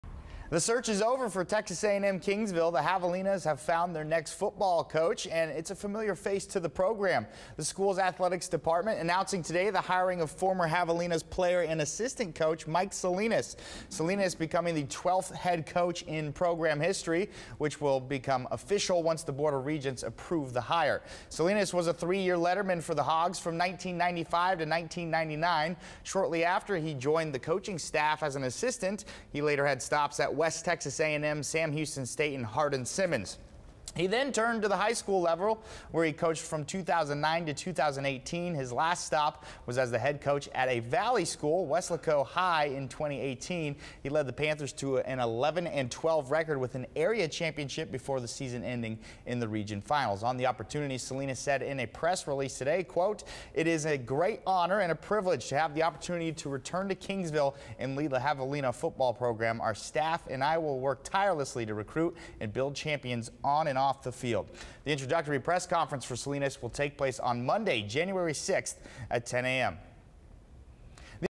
TAMUK hires Mike Salinas as new head football coach
Texas A&M University-Kingsville announced the new head coach for the Javelina football program.